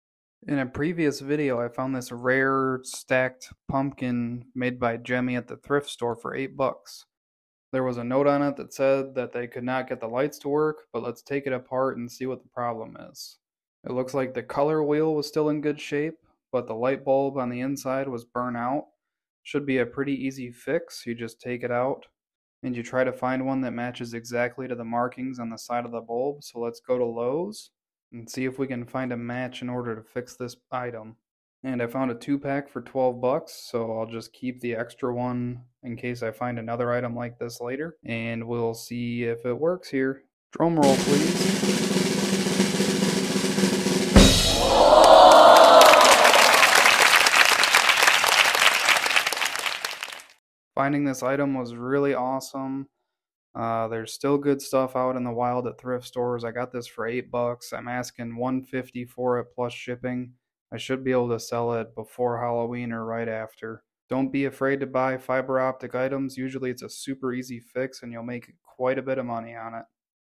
A Rare Gemmy find!
A rare gemmy fiber optic pumpkin. It was broken according to the thrift store, but let’s fix it!!